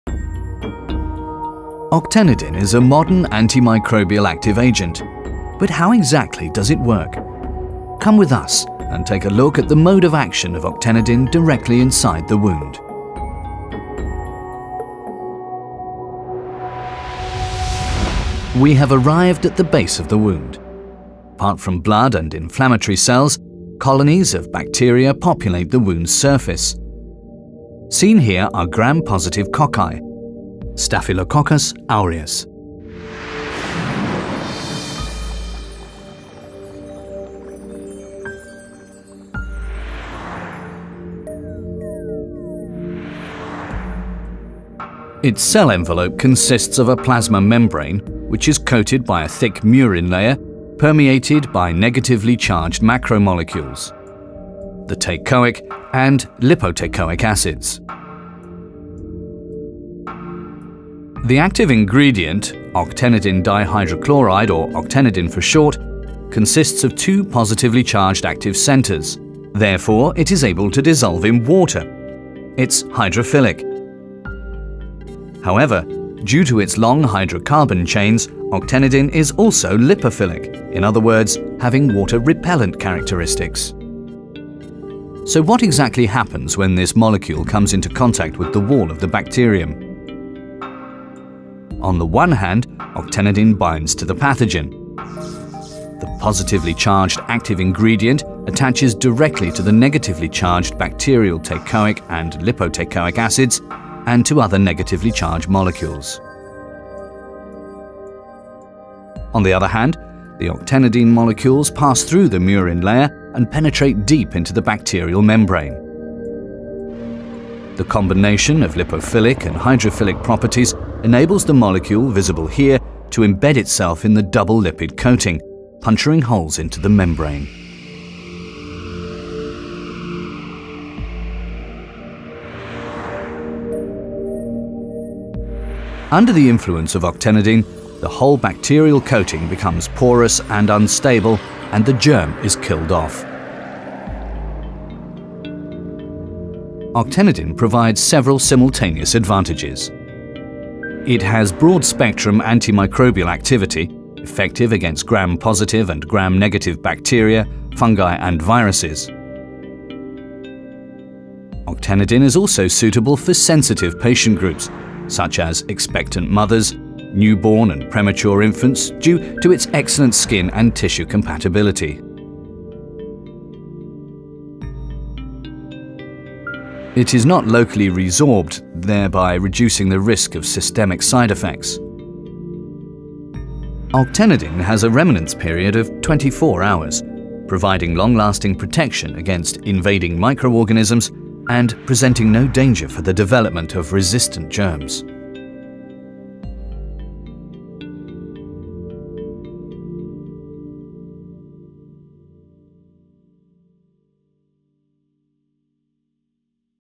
3D Medical Animation - Mode of Action Octenidine
Schülke & Mayr commissioned ANIMA RES to produce a total of four 3D animations on the antiseptic agent, Octenidine. An emotive trailer attunes the viewer to a visual segment illustrating the mechanism of action. Three further 3D animated films on Octenisept® and Octenilin® products containing Octenidine - a wound irrigation solution and wound gel respectively - demonstrate how they can be used.